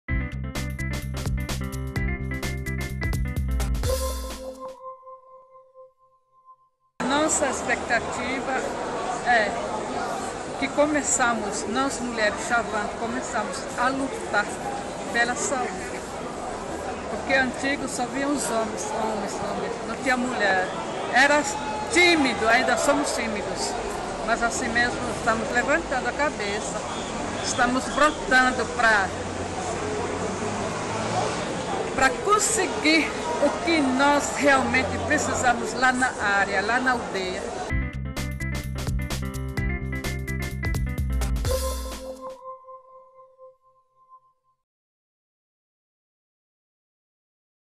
Índia xavante comemora presença feminina na luta por melhorias
SAÚDE INDÍGENA - 03.12.13: Acontece em Brasília, até 6 de dezembro, a 5ª Conferência Nacional de Saúde Indígena (5ª CNSI). Nesta edição, participam cerca de 2 mil pessoas entre delegados, convidados, usuários e trabalhadores da Saúde Indígena. A índia xavante Pascoalina Rêtarí Otõrõ Rawê fala da participação das mulheres na realização da Conferência.